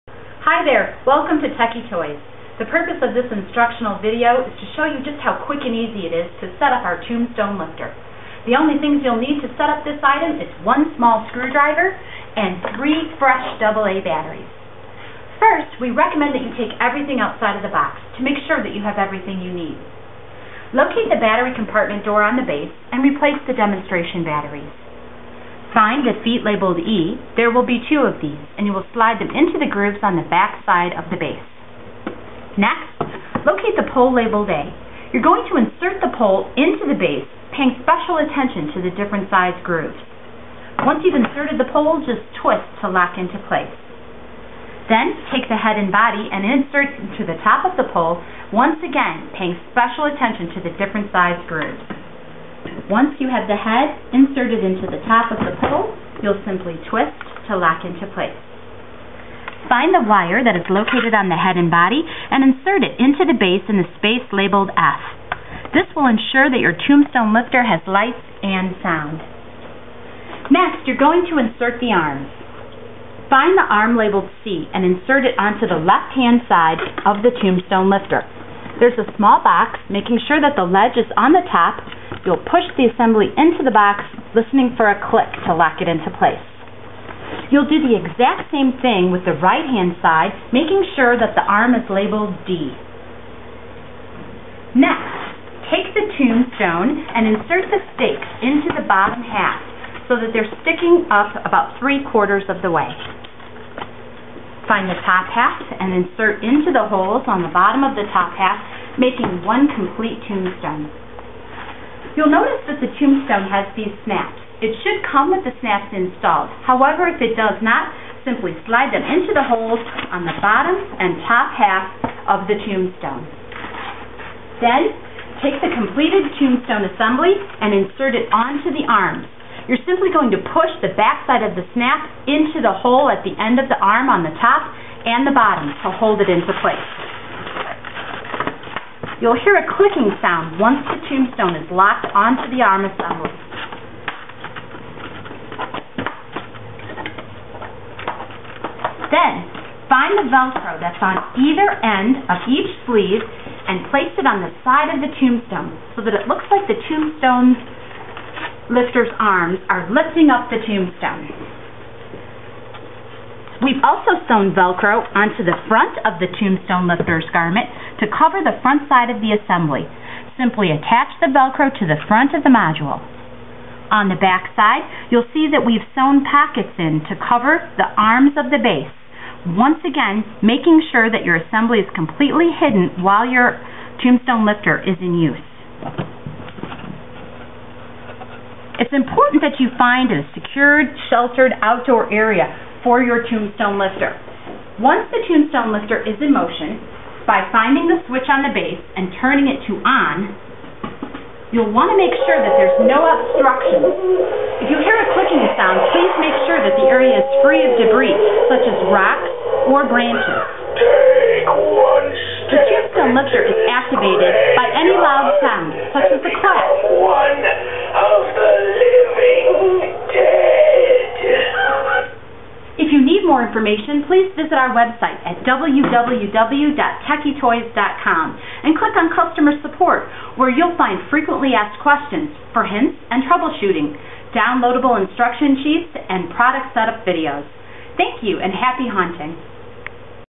Tekky Toys Tombstone Lifter Setup
A quick and easy tutorial on how to set up your Tombstone Lifter.